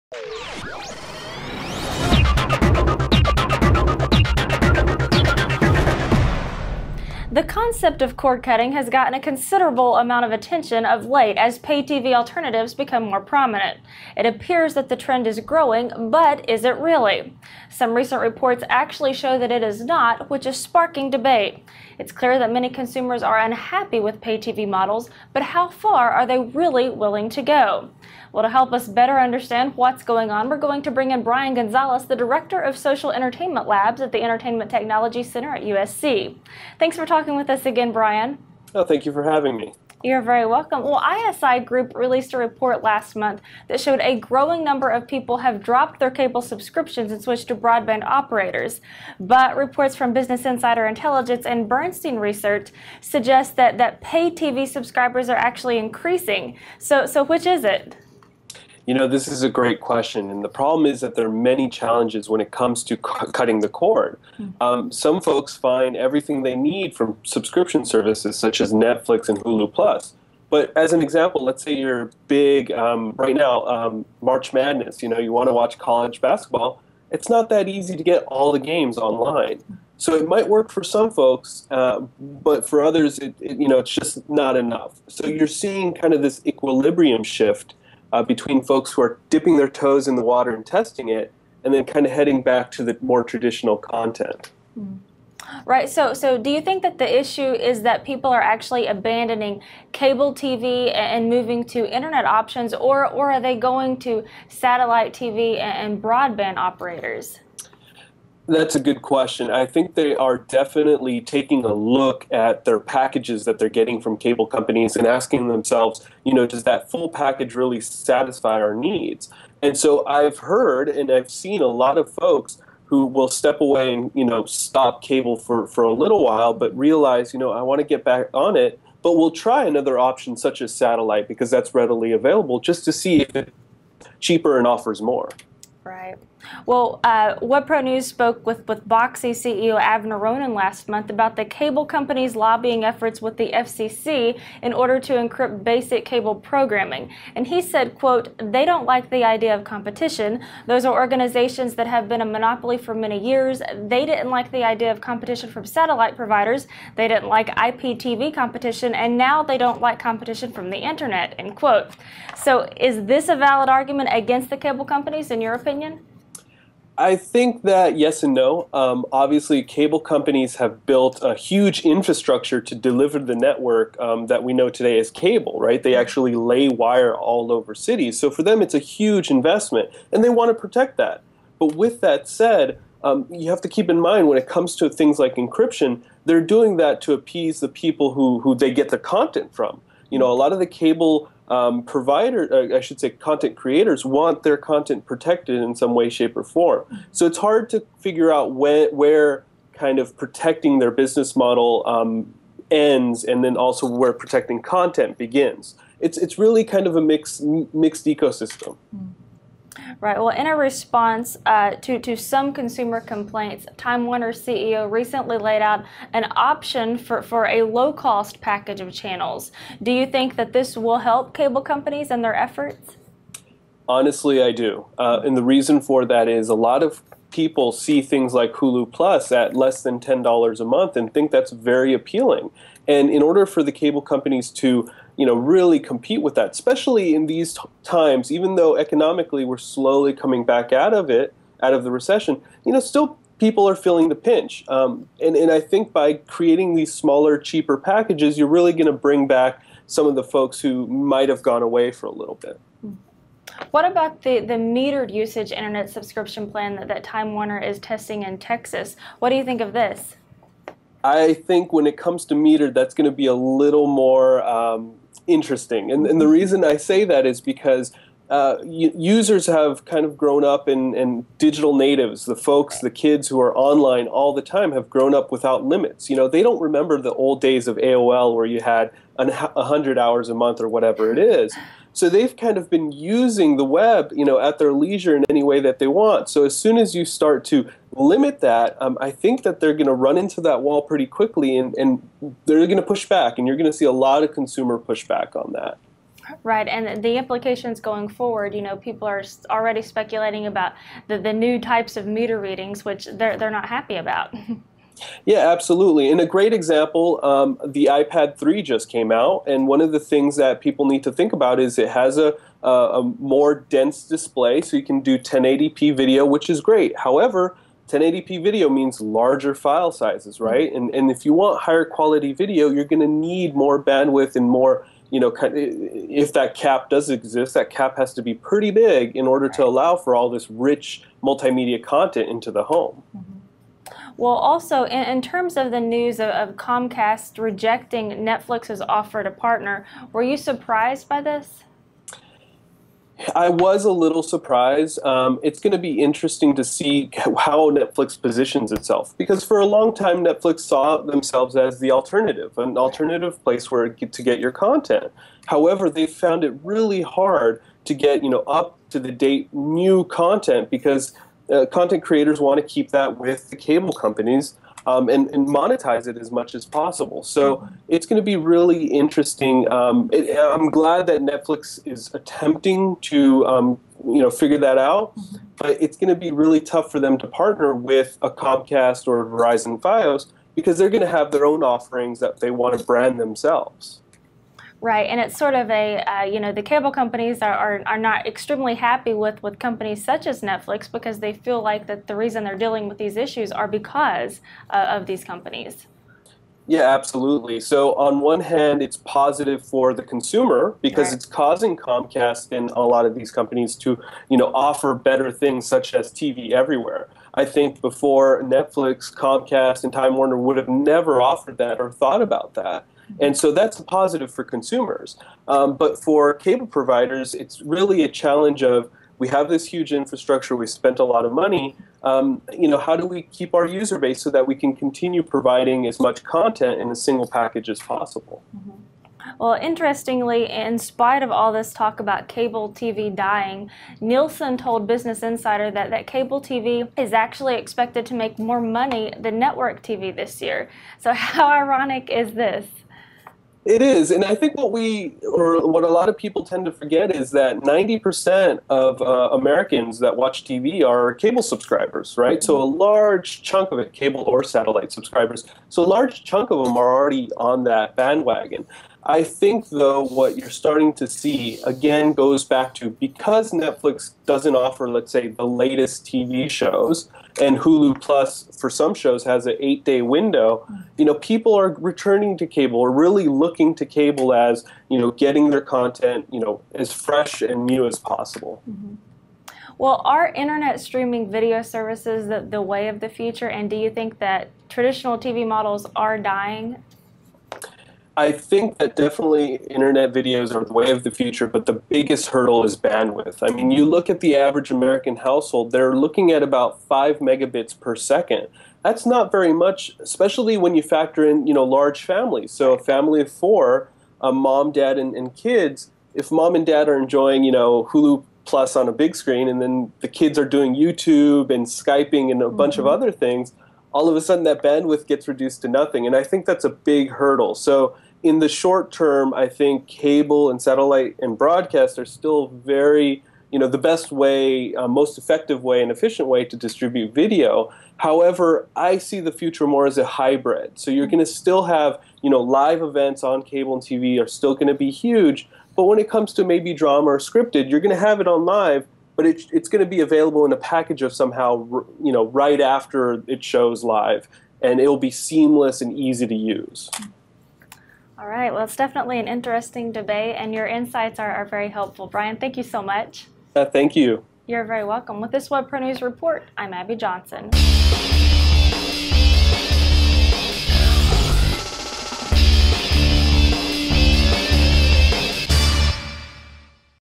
Are Users Really Ditching Cable for Web Alternatives? Analyst Weighs In
The term "cord-cutting" has gained a lot of attention over the past couple of years as more pay-TV alternatives have become available. A report from ISI Group indicates that cord-cutting is becoming a trend since a growing number of people are dropping their cable subscriptions and switching to broadband or satellite TV operators.

However, information from Bernstein Research and Business Insider Intelligence suggests that pay-TV subscribers are increasing. To provide some clarity to the conflicting research, WebProNews spoke with Bryan Gonzalez, the Director of Social Entertainment Labs at the Entertainment Technology Center at USC.

According to him, there are many challenges to cutting the cord. One of the biggest challenges is the lack of bandwidth. He told us that this problem would likely increase as families grow and attempt to bring more rich content into their homes.

Another challenge is the fact that it's difficult to get the most current content through subscription services such as Netflix and Hulu. Gonzalez believes that many consumers have tested the waters with these Internet options but are now going back to more traditional content methods.

Going forward, he told us that Internet services would "definitely" play a big role, but that cable, satellite, and broadcast options would still be prominent. He expects to see a hybrid model at some point in the future.